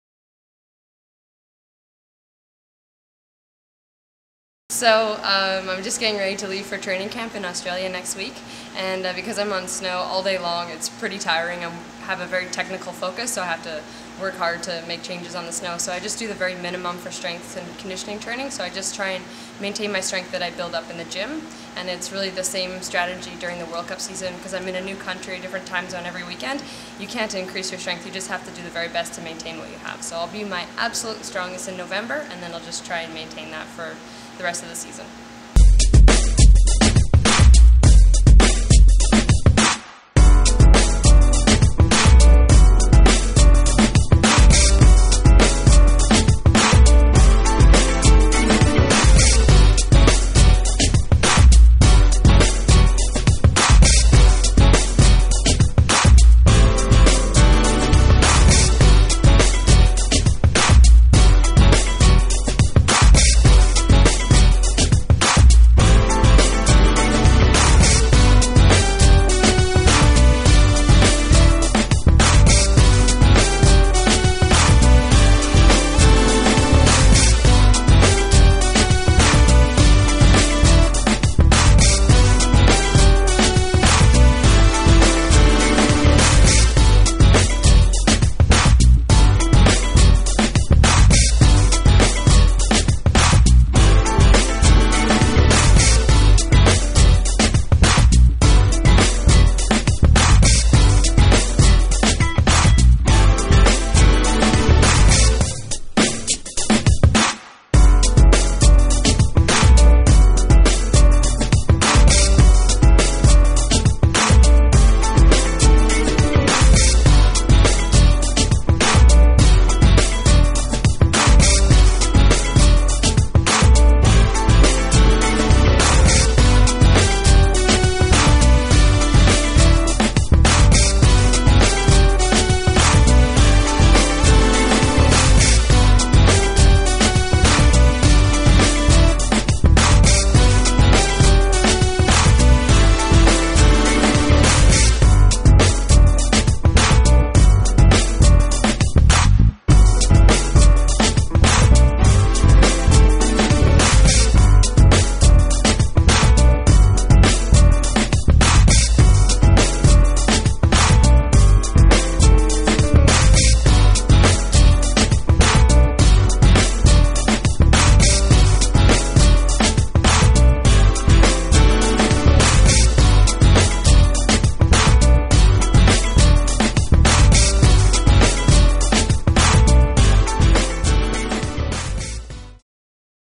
Jenn indoor training at Premier Studio
Watch Jenn as she trains hard for the upcoming moguls season.